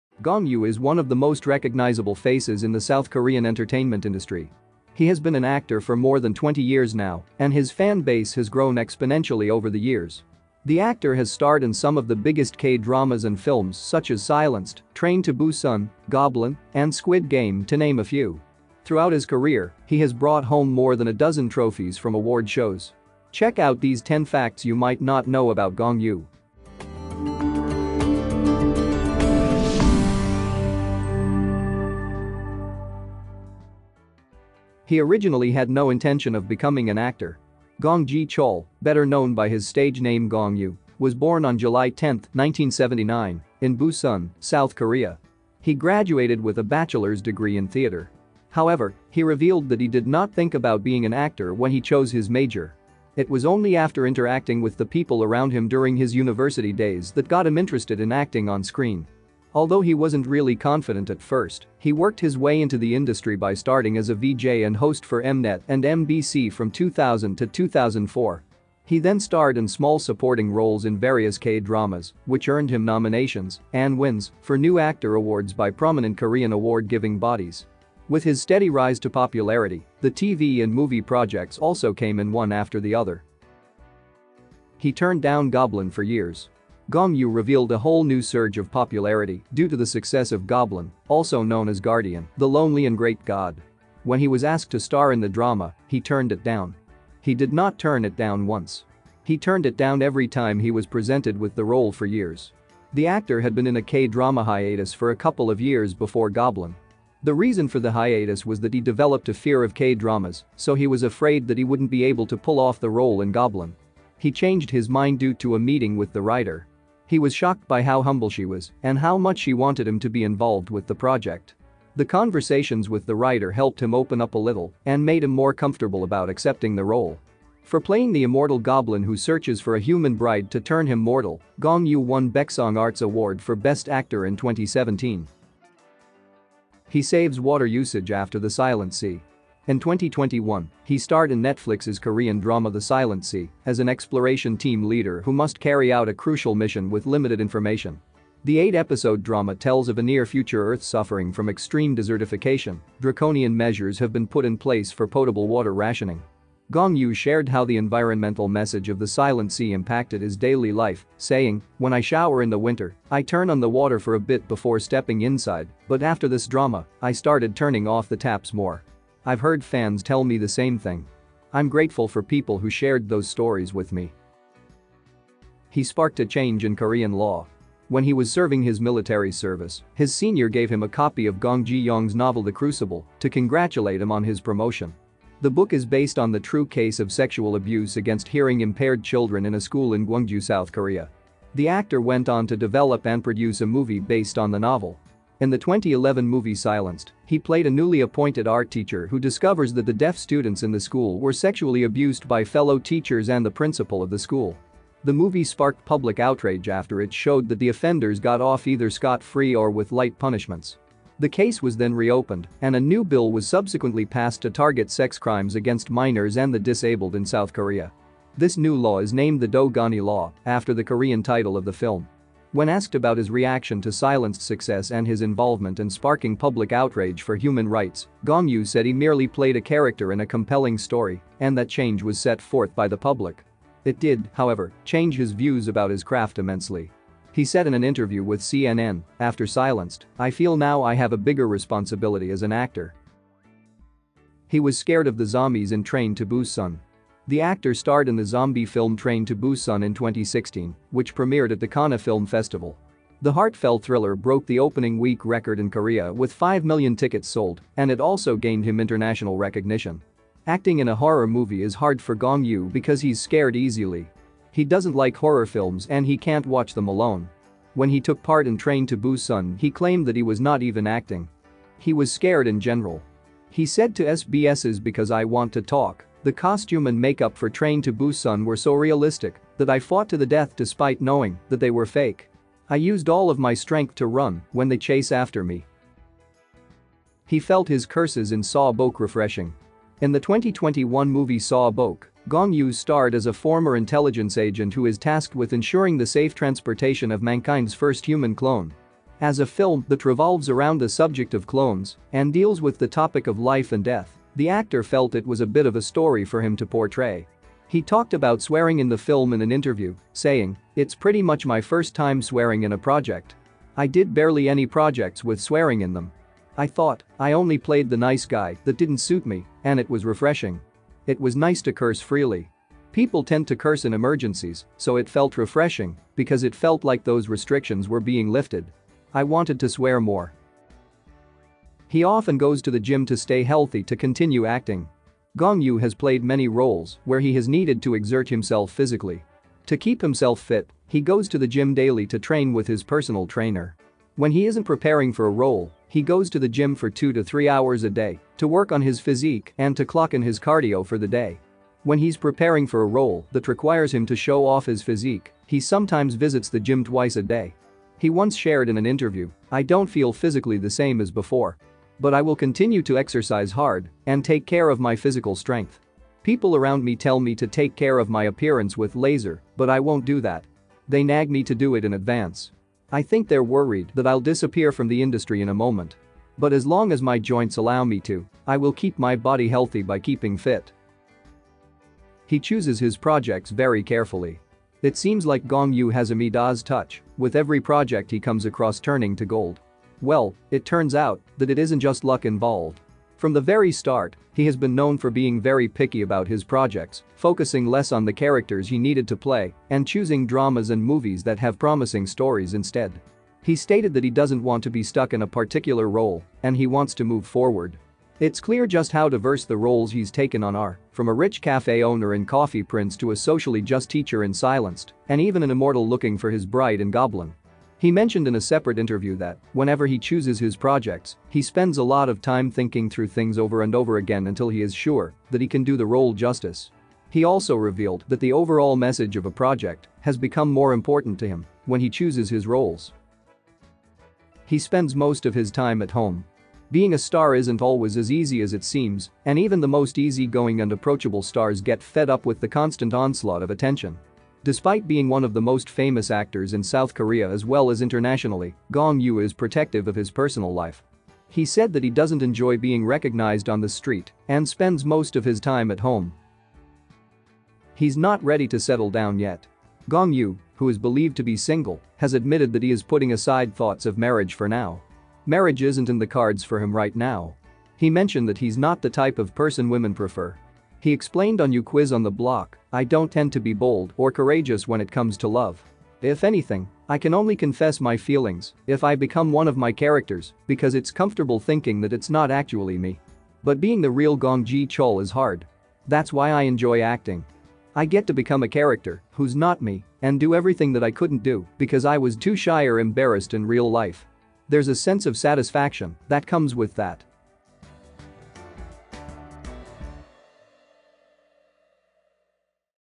10 Things You Didn't Know About Gong Yoo (공유) | Star Fun Facts
Some things you probably didn't know about Gong Yoo (공유)